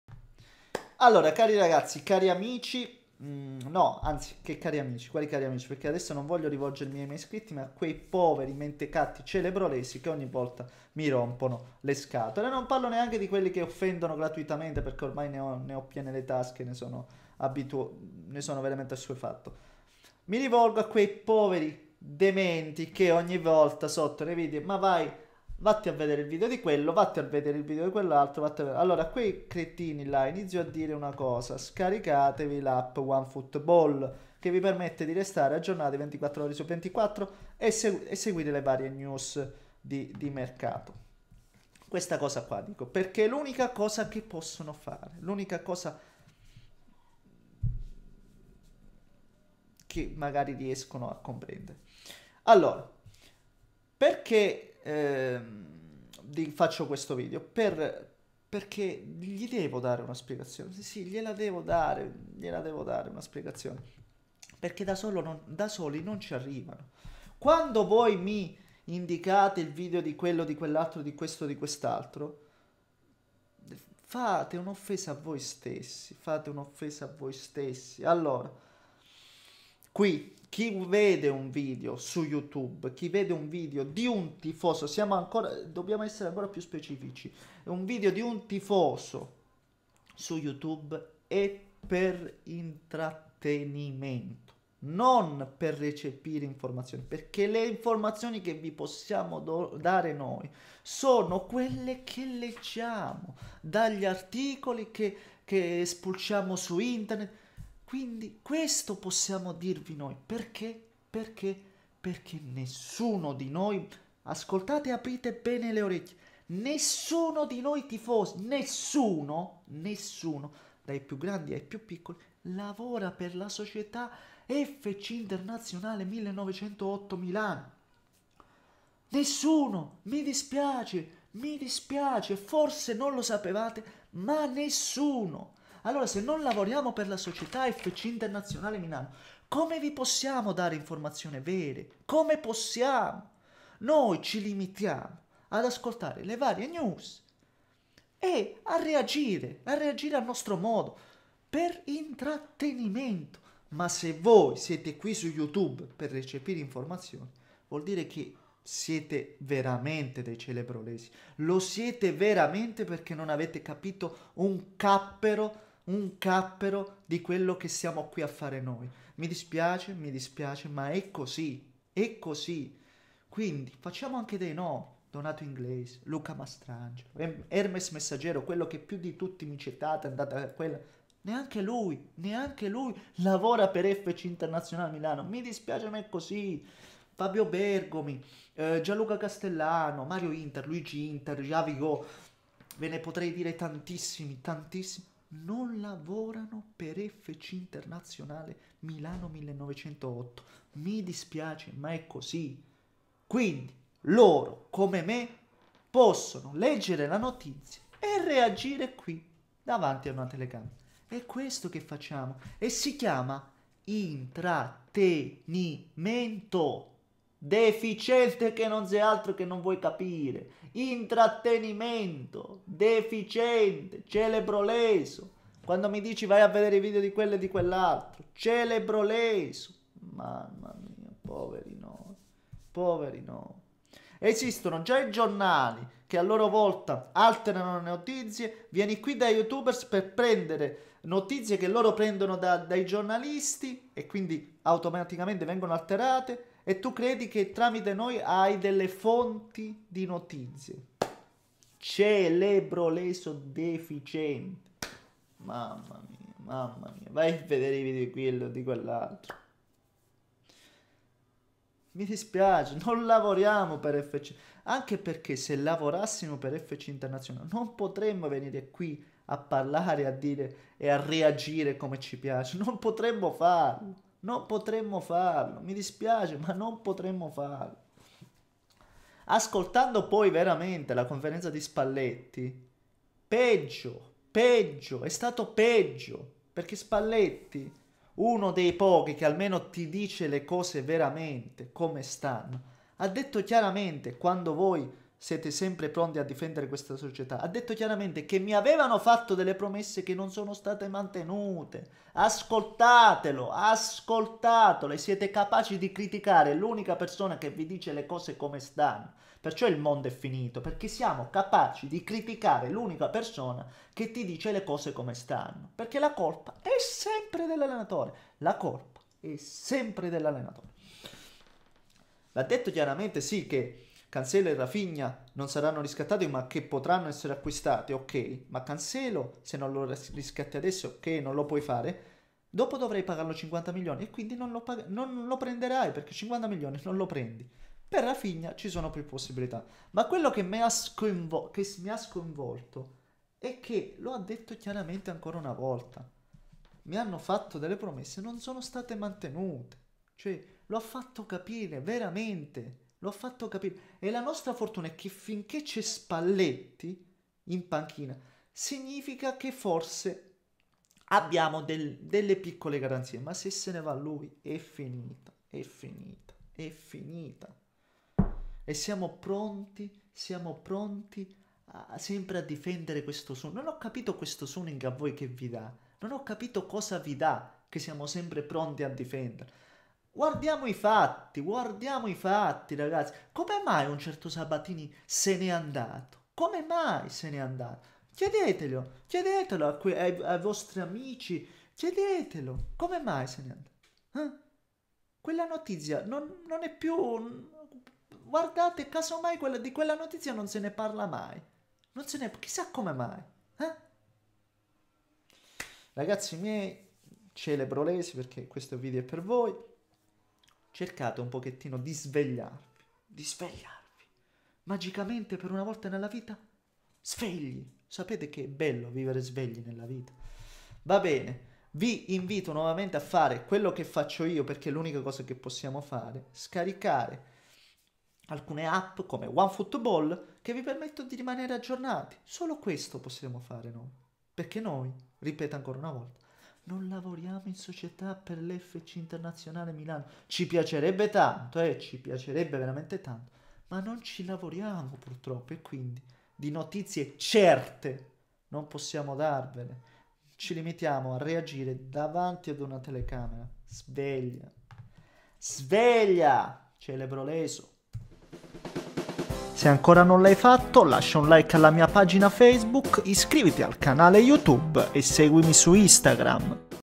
Cancelo e Rafinha? VIDEO PER I CEREBROLESI
Sul mercato le idee sono chiare: "La società si è già mossa bene ma non ricordiamoci che alla fine i conti devono tornare: c'è un settlement agreement da rispettare, firmato dalla proprietà precedente. Noi, avendolo ereditato, dobbiamo essere bravi a non sforare questi paletti. Cancelo e Rafinha in questo momento non si possono riscattare, vi anticipo io: in un secondo momento vedremo. Sono due giocatori importanti ma la situazione è questa. Per un loro acquisto più in avanti la vedo dura, servono parecchi soldini..."